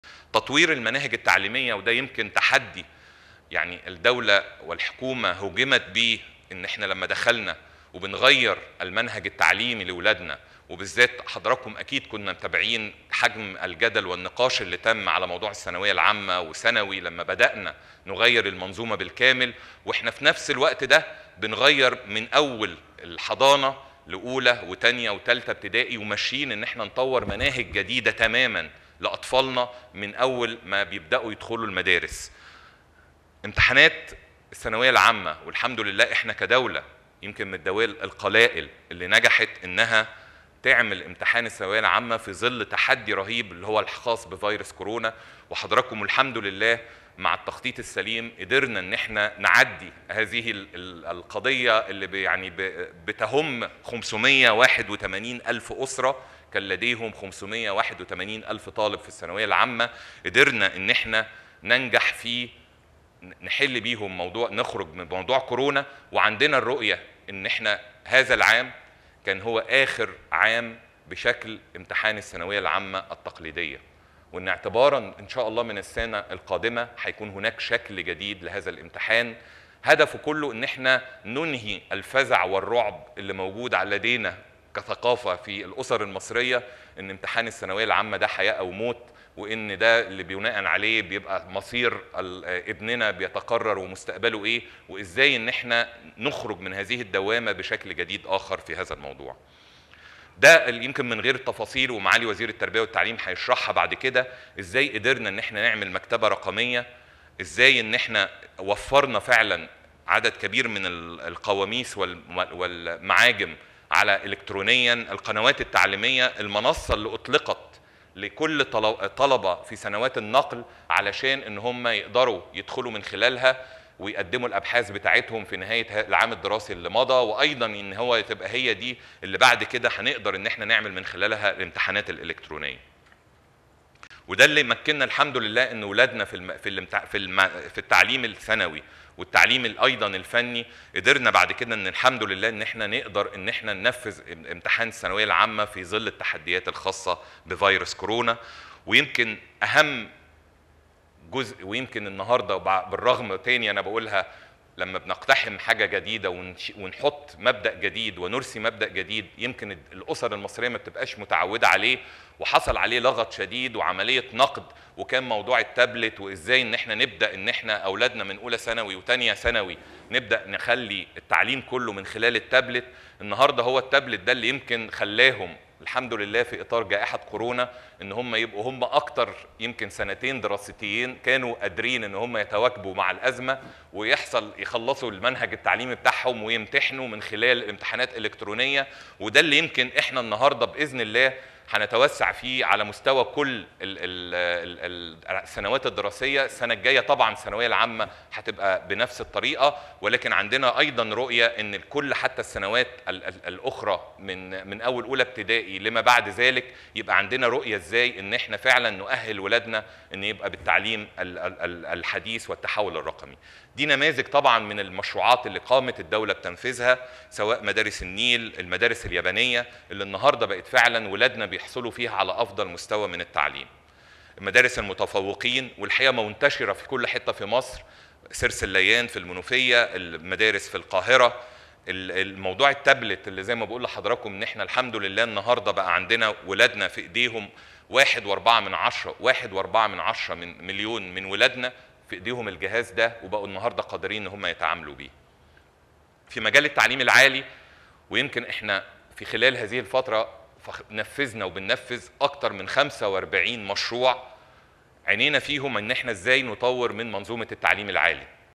الآن | رئيس الوزراء: مصر من الدول القليلة التي نجحت في إتمام امتحانات الثانوية العامة في ظل كورونا
الآن | رئيس الوزراء: مصر من الدول القليلة التي نجحت في إتمام امتحانات الثانوية العامة في ظل أزمة كورونا
16  سبتمبر 2020